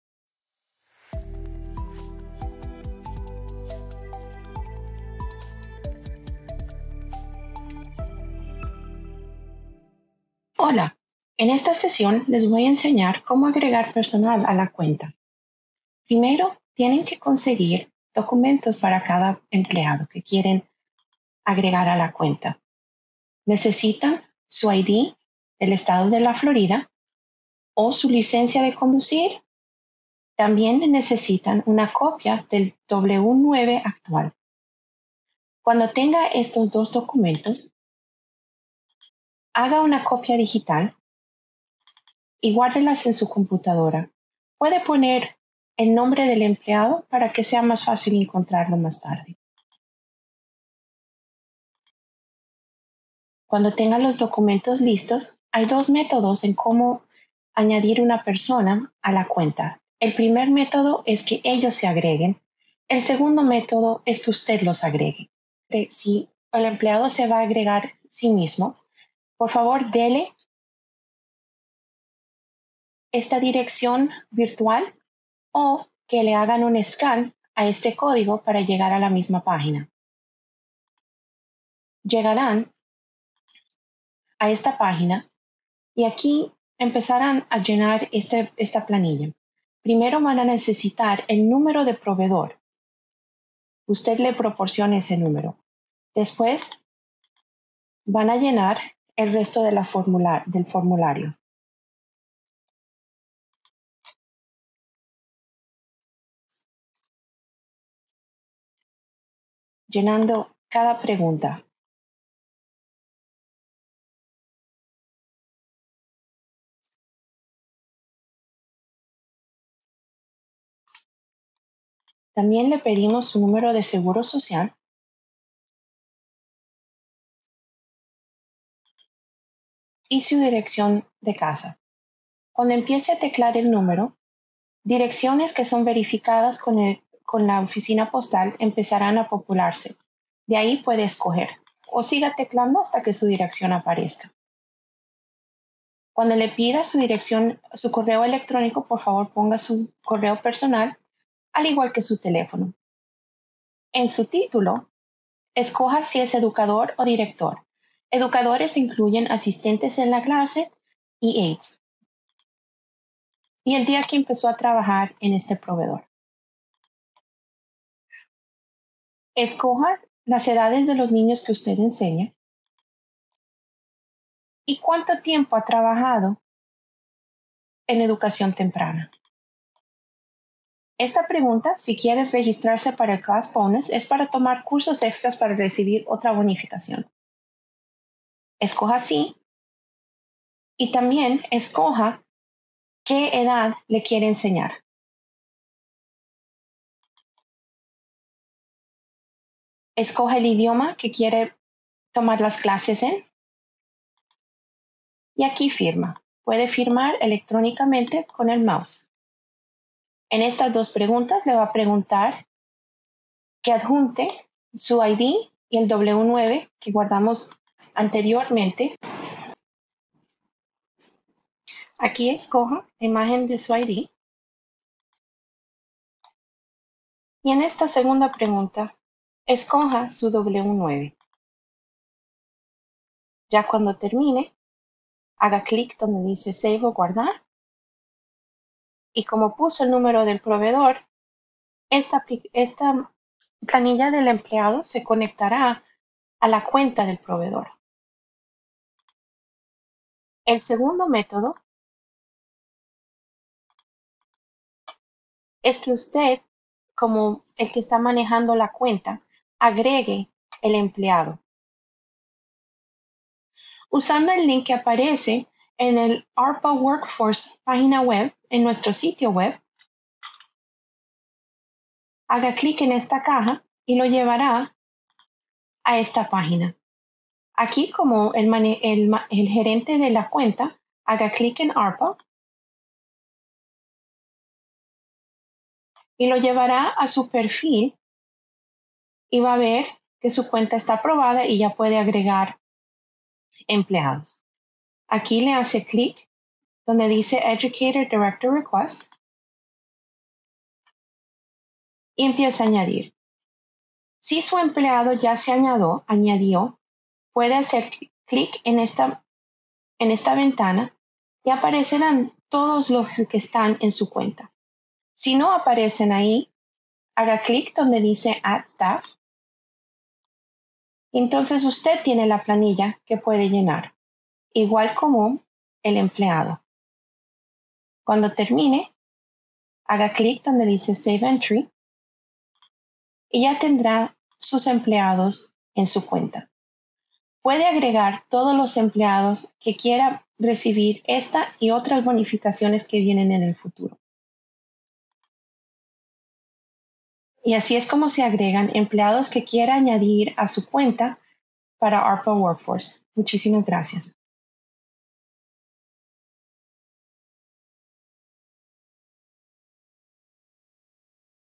Agregando Personal a la Cuenta
ARPA Build a World Class Workforce.
Este tutorial le mostrará cómo agregar personal a su cuenta.